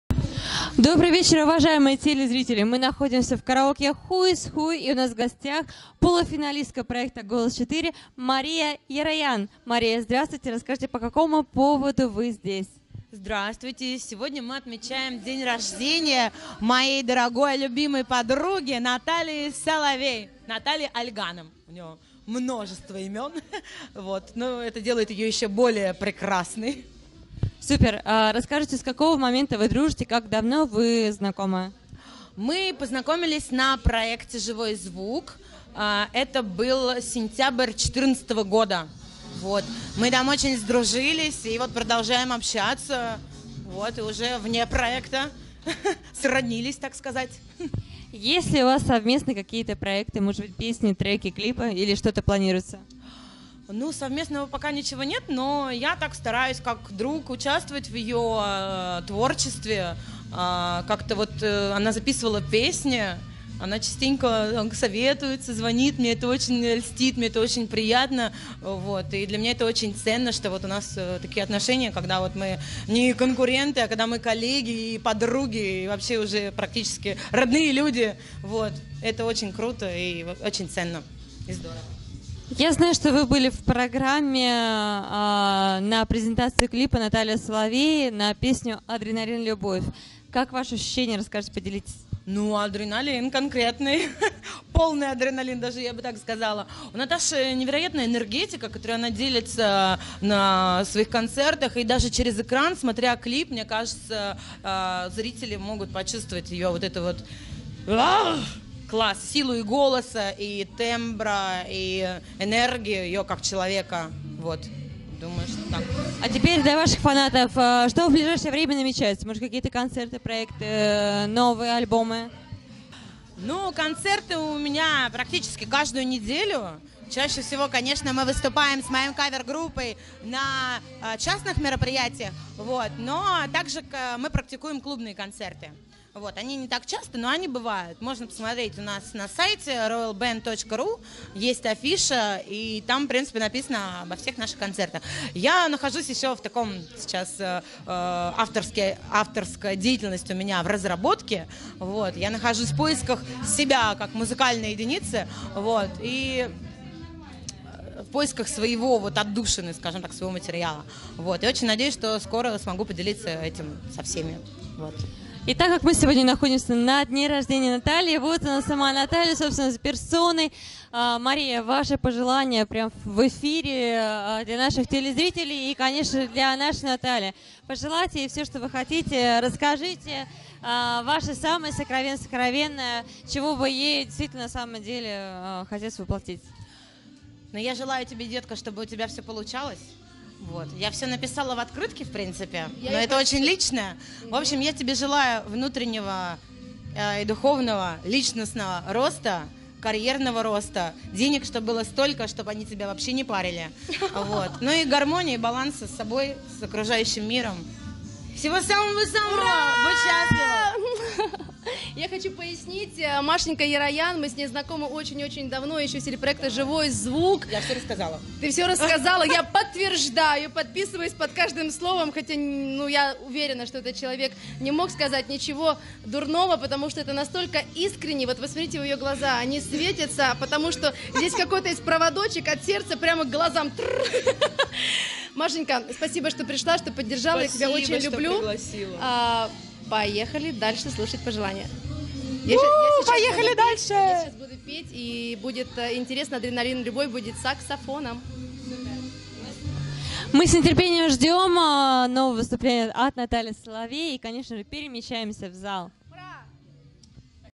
Интервью Марии Ероян и Натальи Соловей -"TV SHANS"
Мария Ероян и Наталья Соловей ответили на вопросы корреспондента студии "ТВ ШАНС" Виктории , на дне рождения певицы Натальи Соловей, в караоке клубе "Ху из ху", на Арбате.
Так же во время интервью прошла рекламная акция уникального препарата для здоровья "ТРЕГИ -Ф"